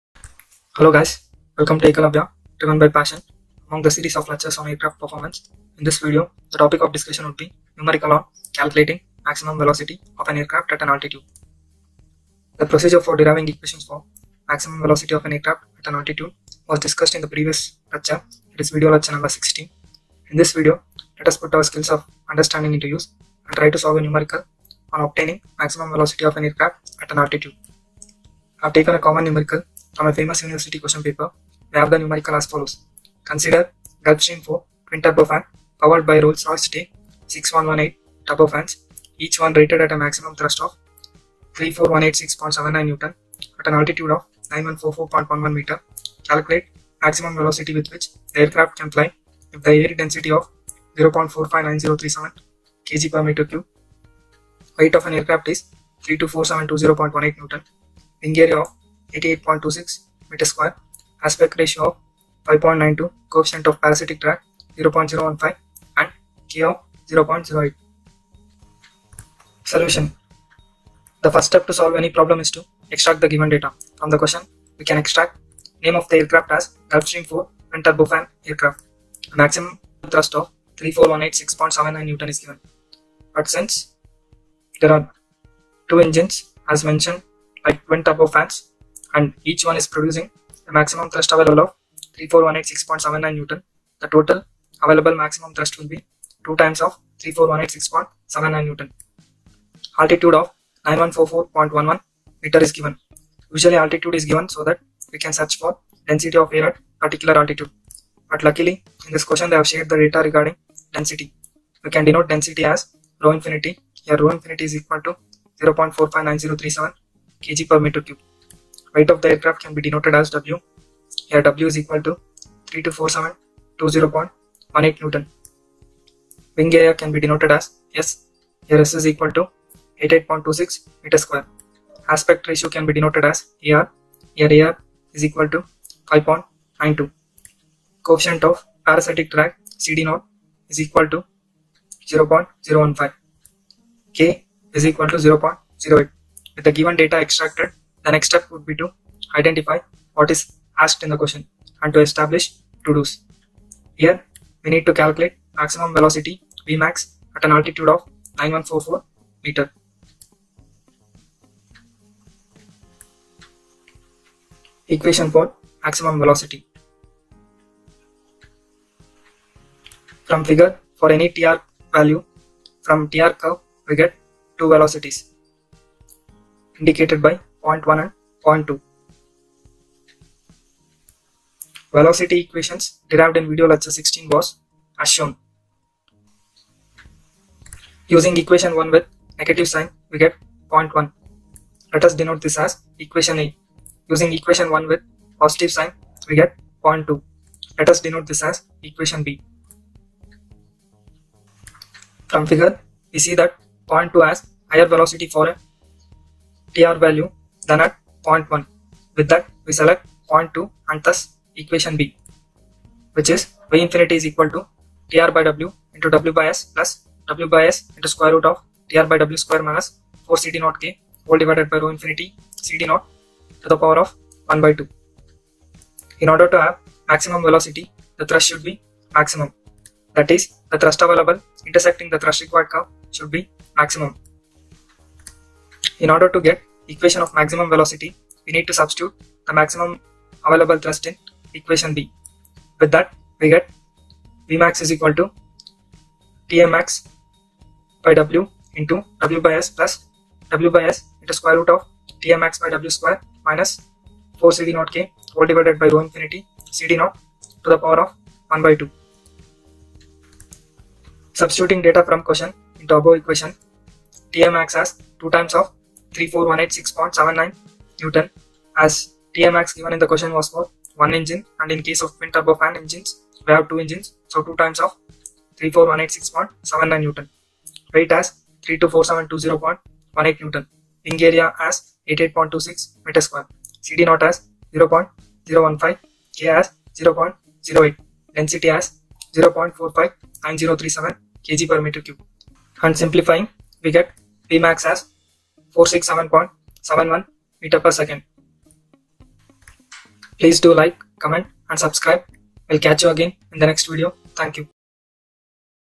AIRCRAFT PERFORMANCE ||(Analytical Approach) Numerical on Calculating Maximum Velocity ||Lecture #18
When an Airplane pass over in the sky, have you ever wondered,
How fast an airplane can fly?
How far an airplane can fly?
How long an airplane can fly?
How fast an airplane can turn?
How high an airplane can zoom?
What runway length is required for takeoff?
What runway length is required for landing?
Curious about the airplane and above questions?
In this video you'll learn about solving the NUMERICAL ON CALCULATING MAXIMUM VELOCITY of an aircraft using ANALYTICAL APPROACH.